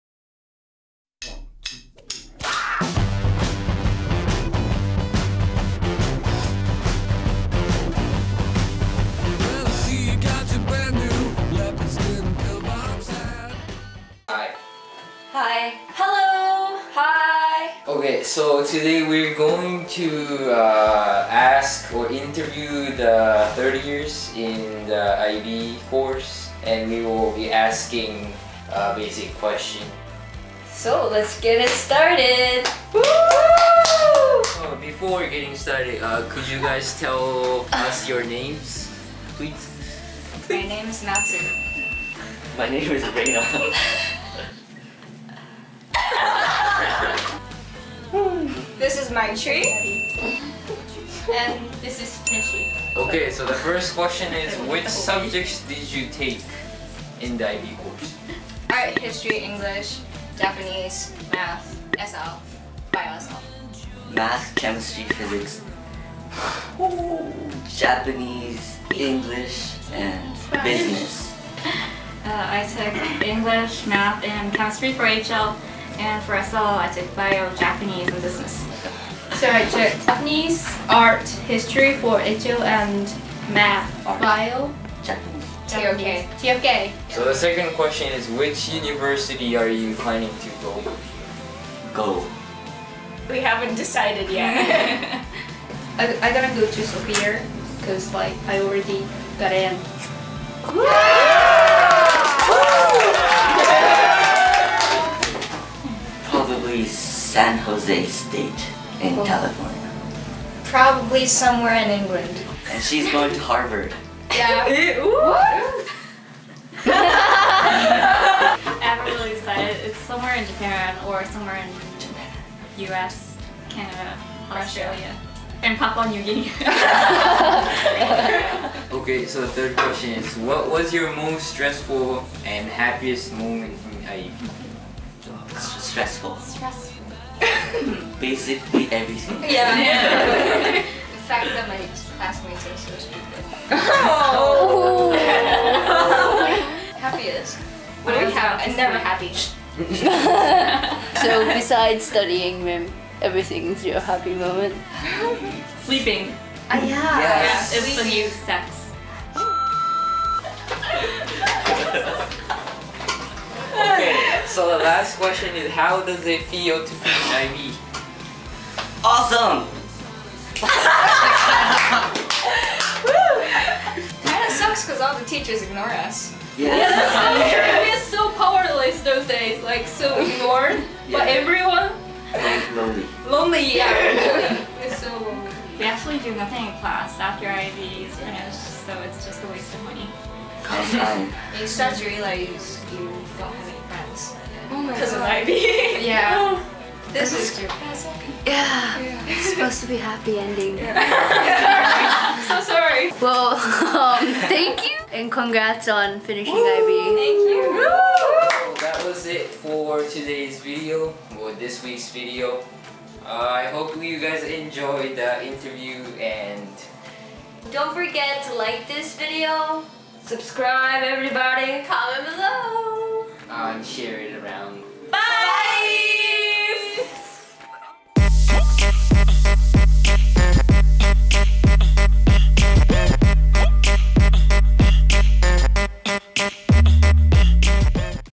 Interview | International Baka
helloo!!

For today's video we've interviewed students that recently finished IB and got their diploma!!
I hope that this video is entertaining and hopefully not a waste of your time! :)

LEAVE ANY QUESTIONS YOU HAVE FOR US BECAUSE WE WILL BE ANSWERING THEM IN OUR FUTURE VIDEOS!